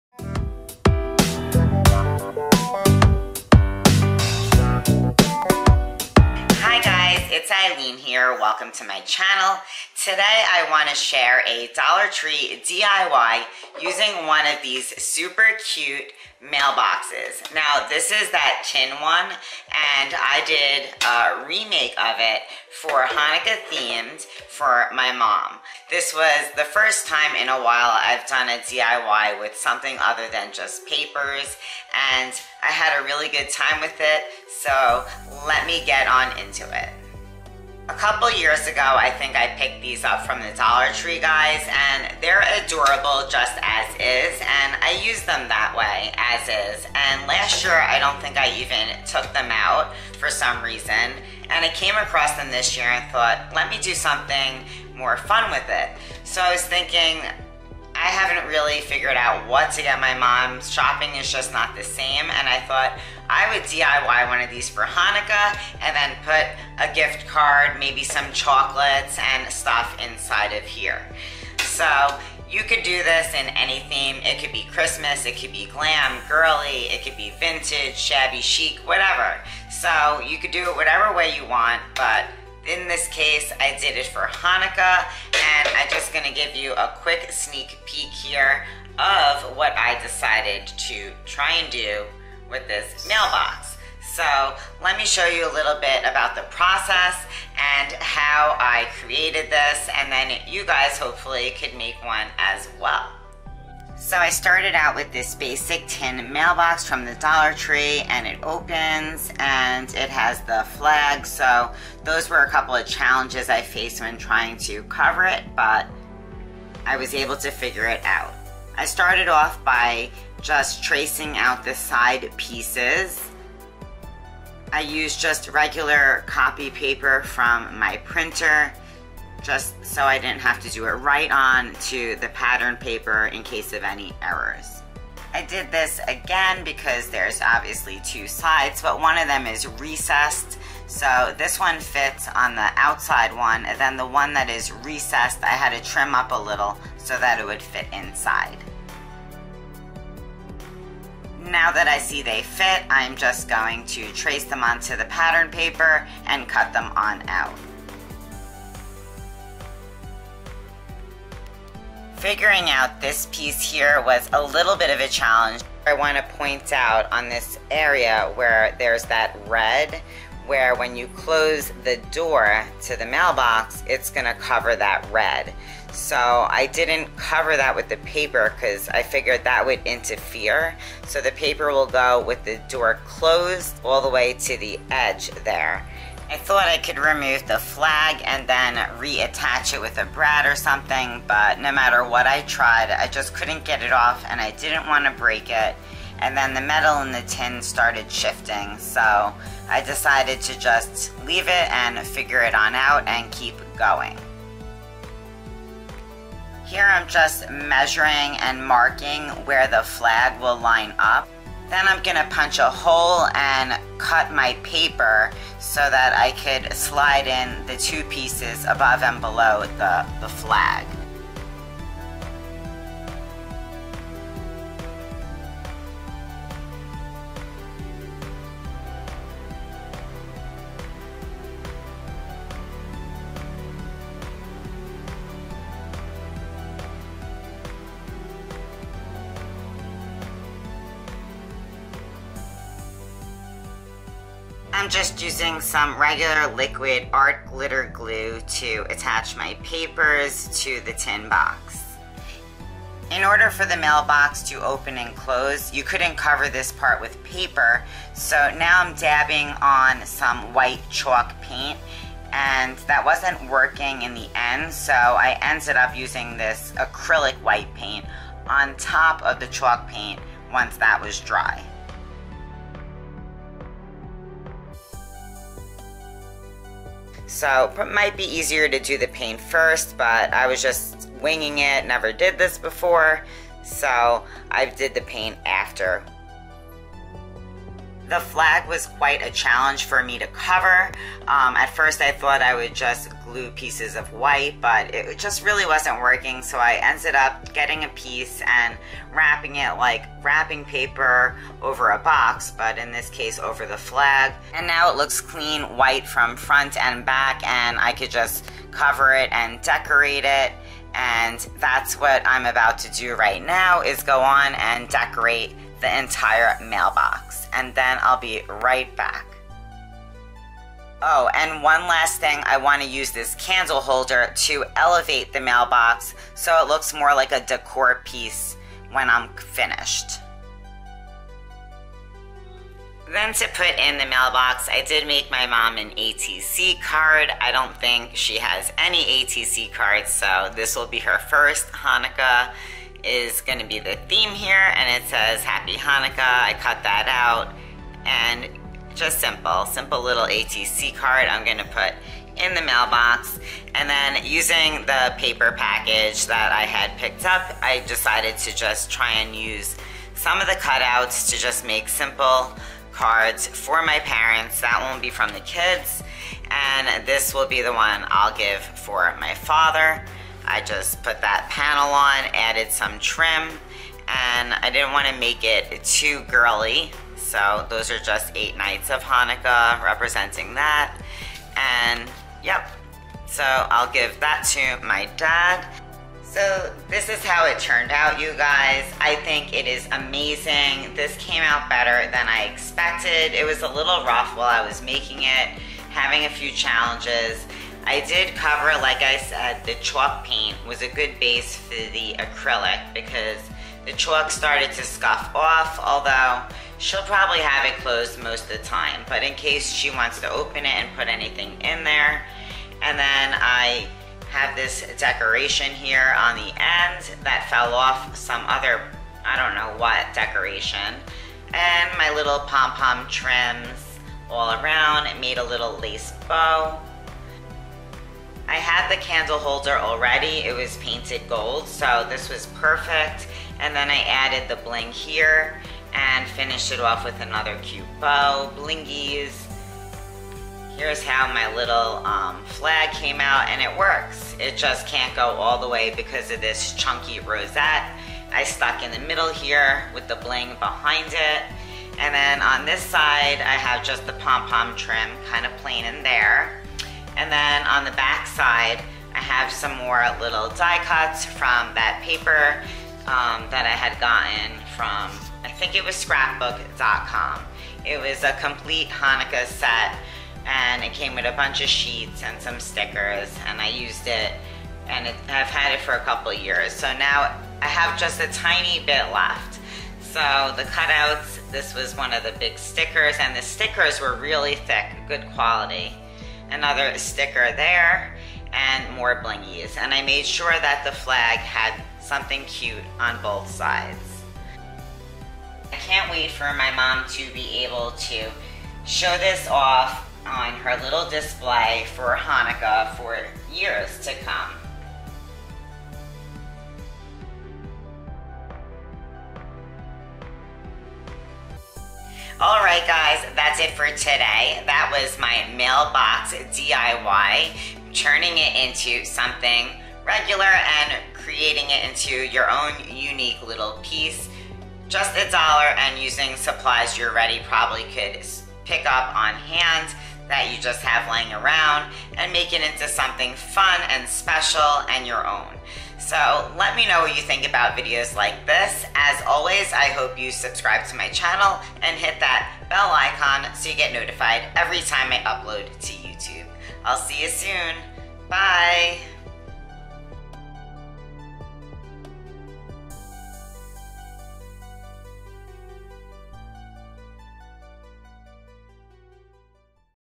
DOLLAR TREE TIN MAIL BOX REMAKE/DIY
I re did the Dollar Tree tin mailbox and transformed it into a Hanukkah decor piece for my mom.  It still opens and is a great way to pack up gift cards, candies or any small gift items.
If you want to swap or collaborate my info is below!

**This video is not for children; for adults ONLY!  **